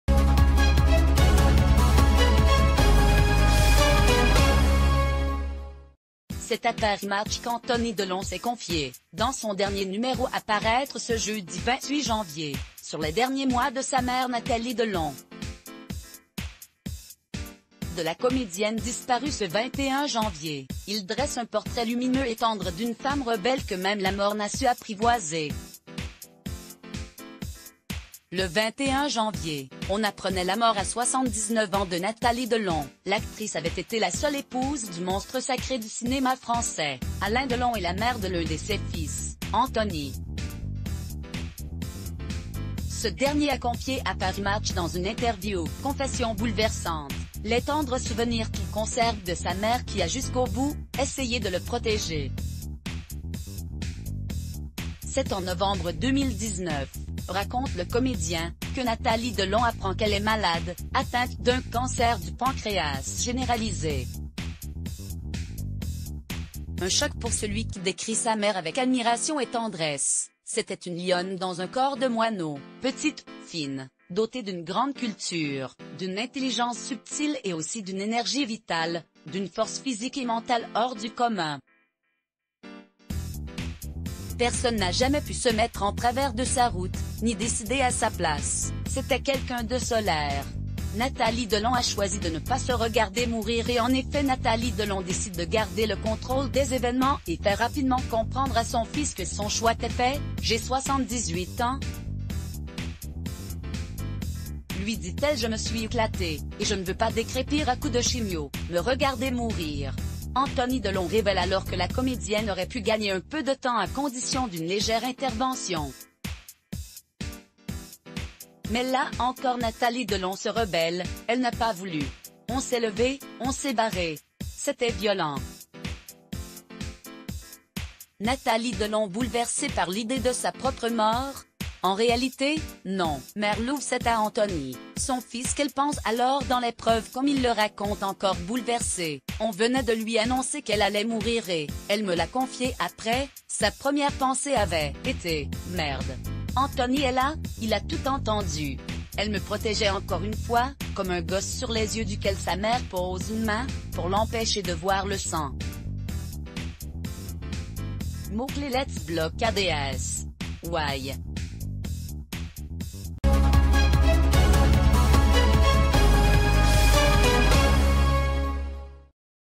Mort de Nathalie Delon : son fils Anthony raconte sa mère à l'épreuve de la maladie - Voici
C'est à Paris Match qu'Anthony...